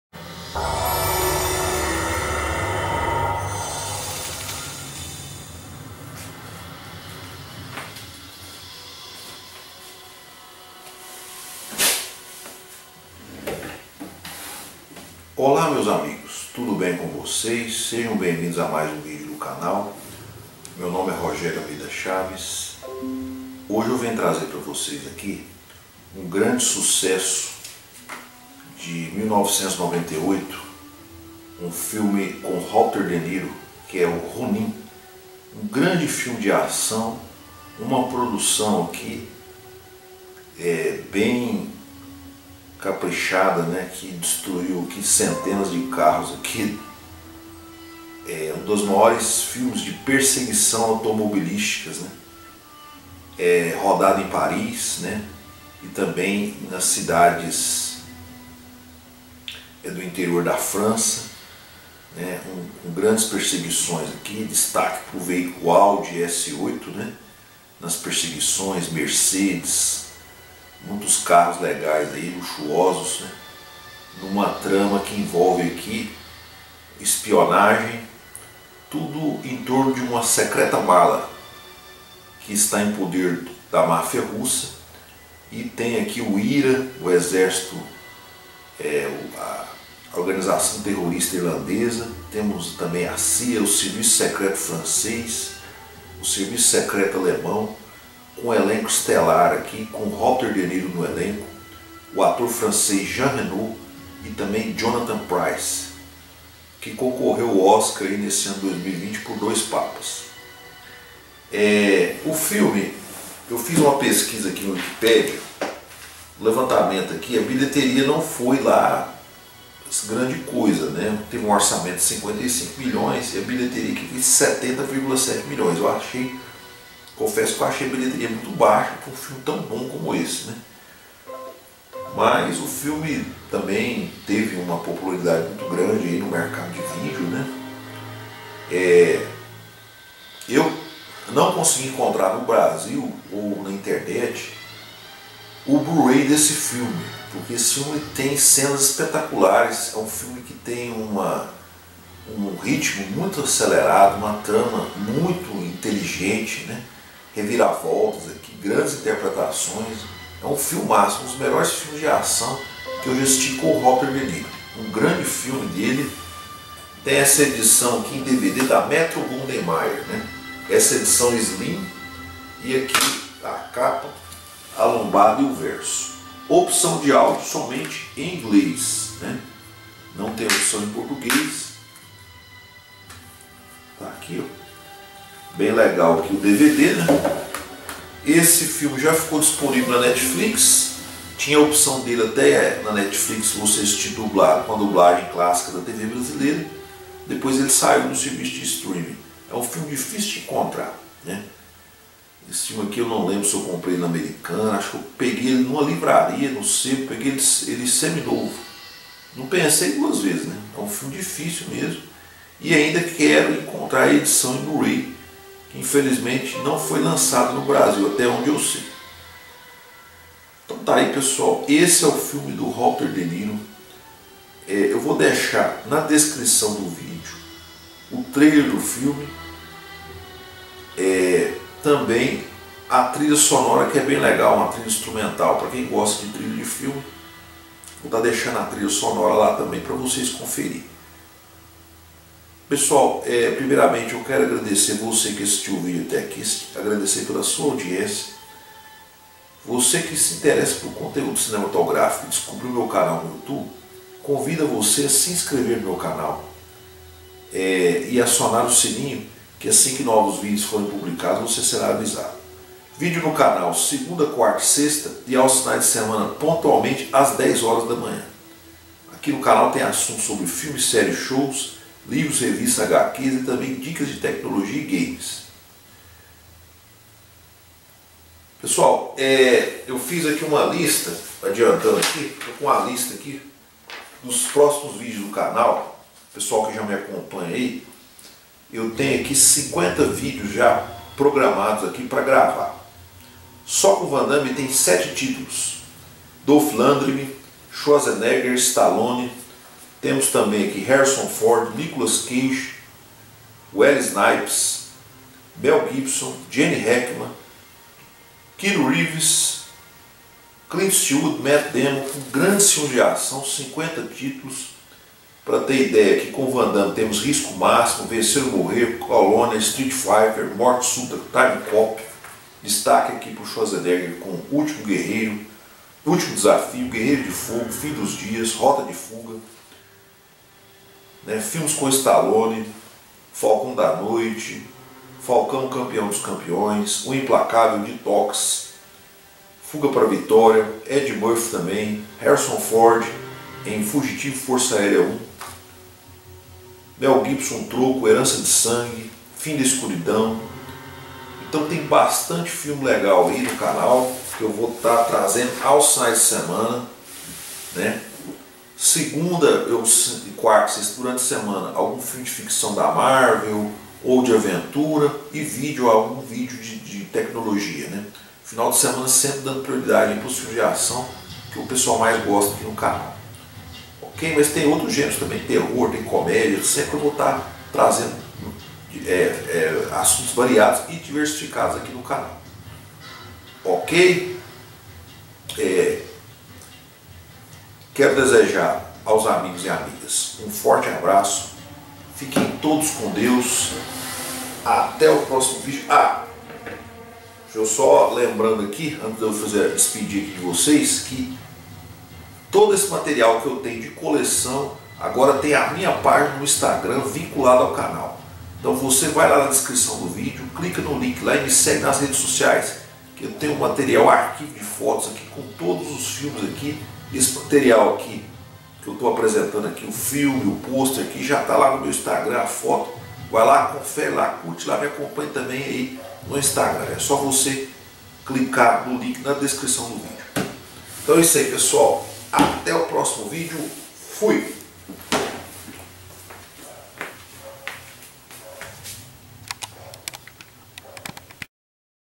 RONIN análise do filme de Robert De Niro
Olá, meus amigos hoje vamos falar sobre o grande sucesso de 1998, o filme RONIN com Robert De Niro liderando um elenco de estrelas, foi uma produção que destruiu dezenas de carros, cenas espetaculares, filme muito bem dirigido.